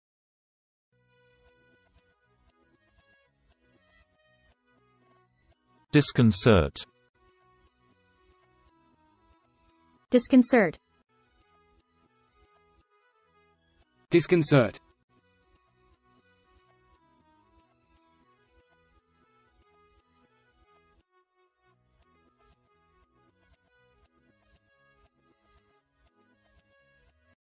Disconcert - How to Pronounce Disconcert in British Accent, Australian Accent and American Accent
Meaning of Disconcert is Unsettle, Distress

Antonym of the word Disconcert is Calm

Unsettle, Distress, "